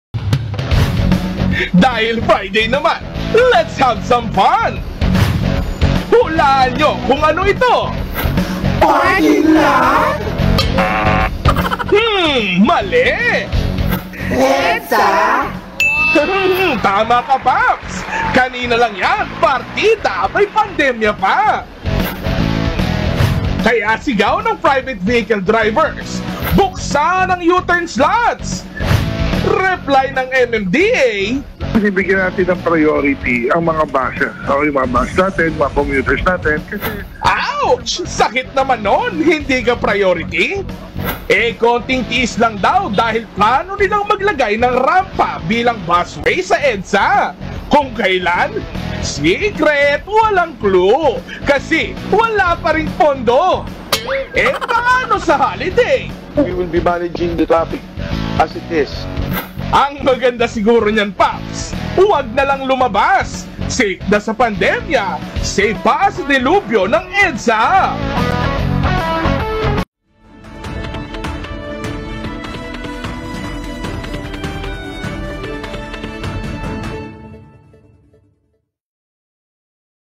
Sitwasyon ng traffic sa EDSA
OneBalitaPilipinas | May tugon ang MMDA sa panawagan ng mga motorista sa isinarang U-turn slots sa EDSA.